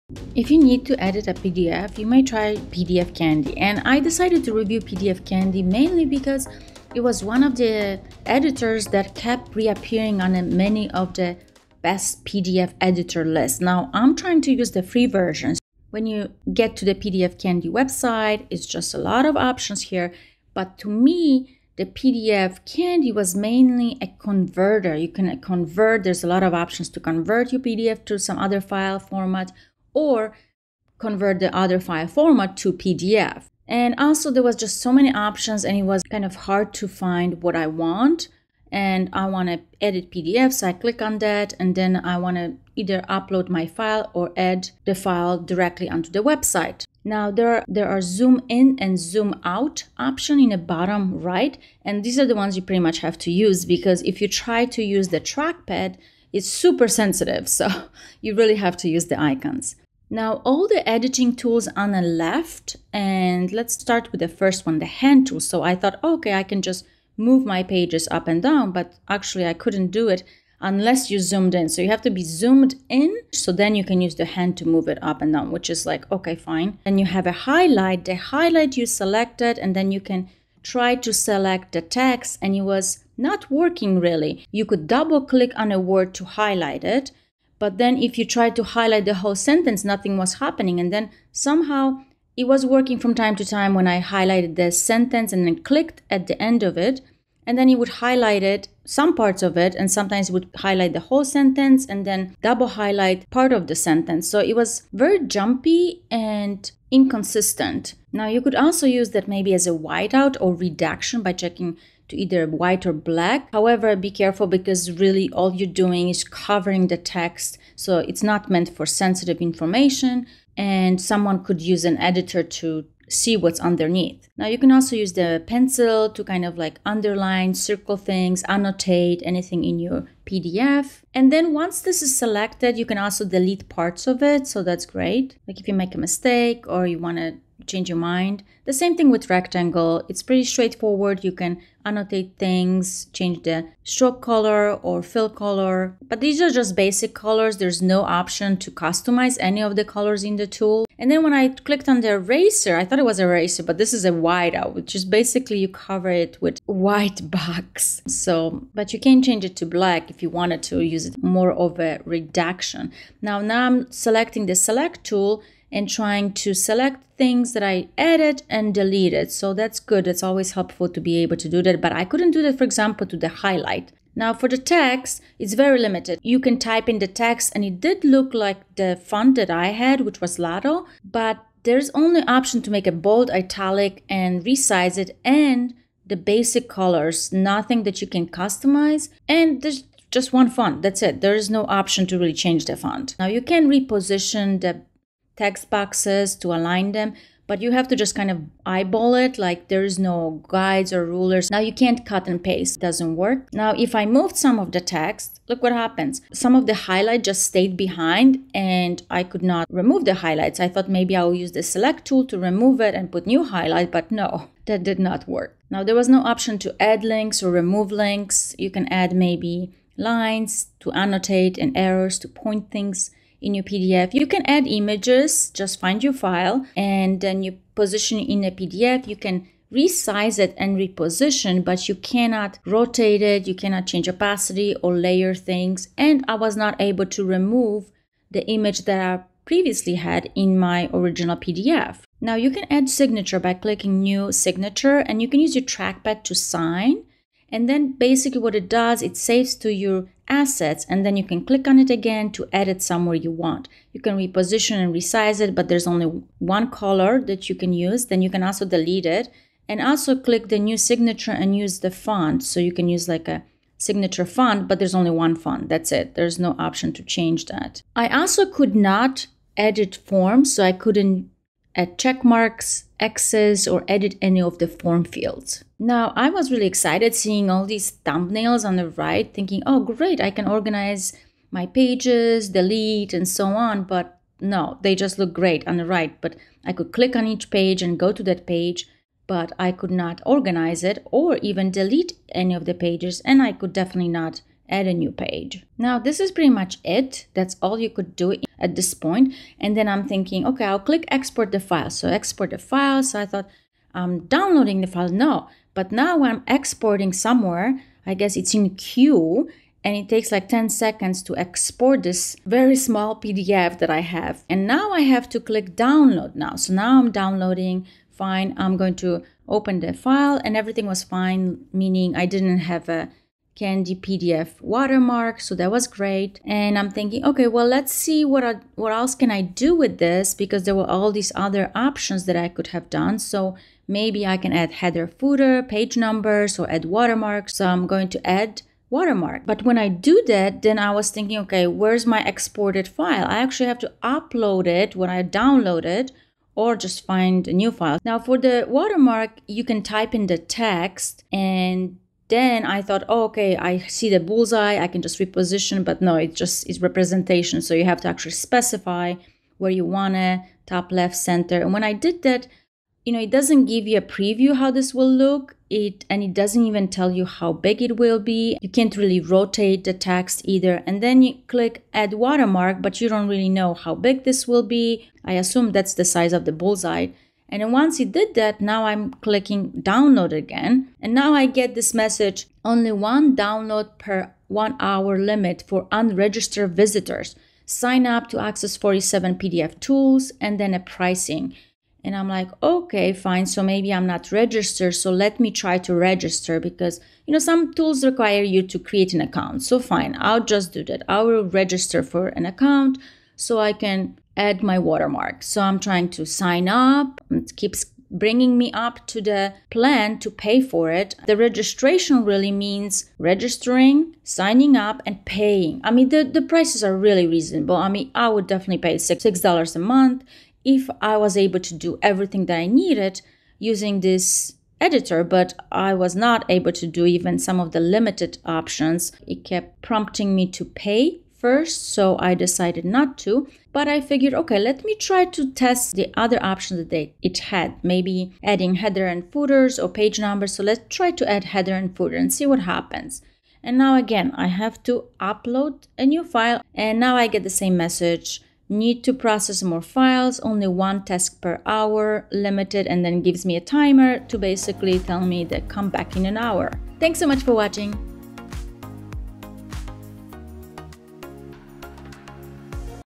How to Use Free PDF Candy Editor Tutorial & Review
PDF Candy offers both a free and reasonably priced $6 per month version of its online PDF editor. However, the free version can be limited and inconsistent in performance, with one font option and limited color choices for text.

It lacks advanced features like form field editing, link management, and page organization, making it more suitable for basic PDF tasks. While it has a range of PDF conversion options, it functions more as a converter than a full-fledged PDF editor, and users may encounter limitations on the number of tasks they can perform in a given time frame.

YES - Annotation
YES - Adding Images
YES - Adding Signatures
NO - Organizing Pages
NO - Form Editing
NO - Little Customization
NO - 1 Font